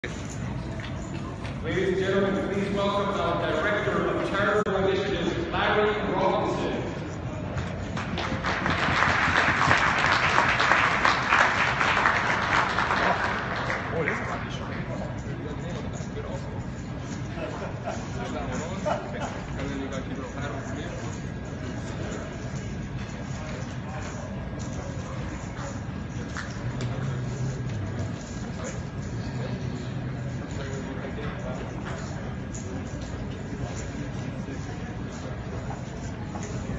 Larry Robinson at Playground
Larry Robinson is introduced at the All-Stars for Starlight Day 1a